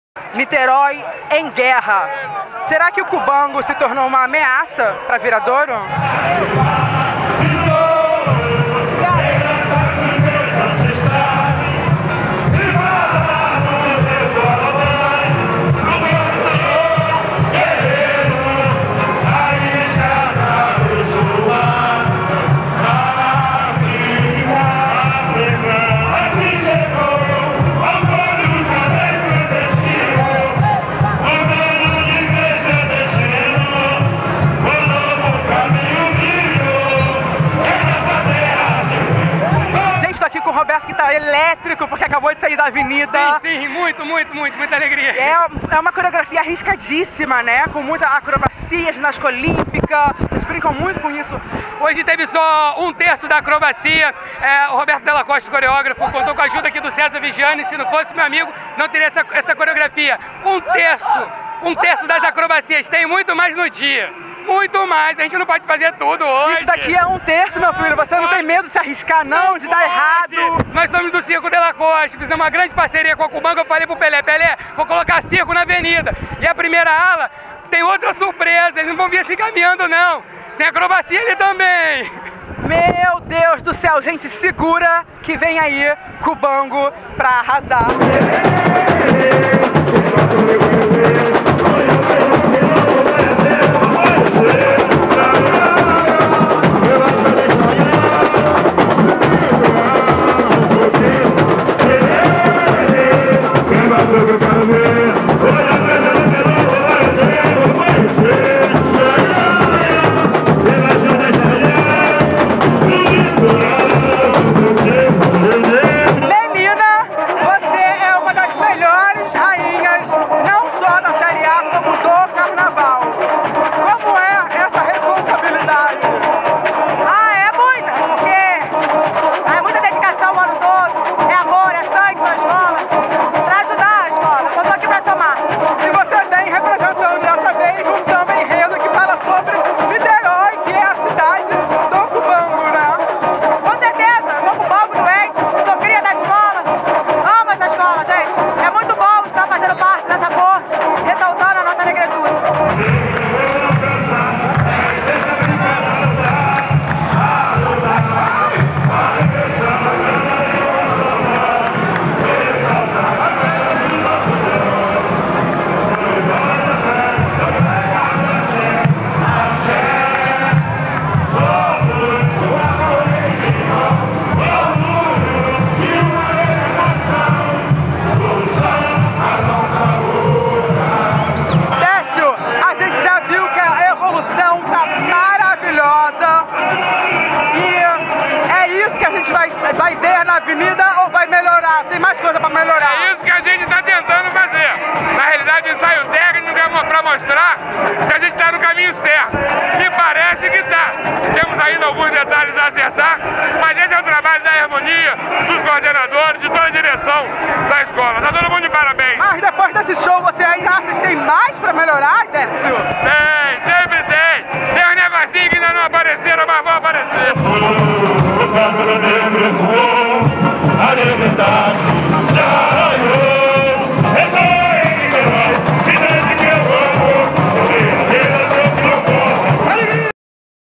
Ensaio Técnico 2015 || Cubango
O ritual continuou. A Cubango fez um Ensaio Técnico com personalidade e valorizando sua harmonia, que passou com excelência durante o treino da Verde e Branco. Décio Bastos, Diretor de Harmonia da escola, satisfeito com o resultado, afirmou que ainda tem uma coisnha ou outra pra melhorar, assim como Roberto De La Costa, coreógrafo da comissão de frente da agremiação, que com uma coreografia cheia de acrobacias, não escondeu totalmente o jogo, mas o elemento surpresa, segundo o próprio, só será visto no desfile oficial. Mostrado que mesmo sob novo comando, continua Folgada, a bateria de Mestre Maurão foi um dos destaques da noite, mas teve que dividir as atenções com Cris Alves, que com certeza já escreve na história seu nome como uma das melhores rainhas de bateria da Avenida. Com samba, classe e total integração com seus ritmistas, sabe respeitar o que vem apresentando. Realeza de Niterói.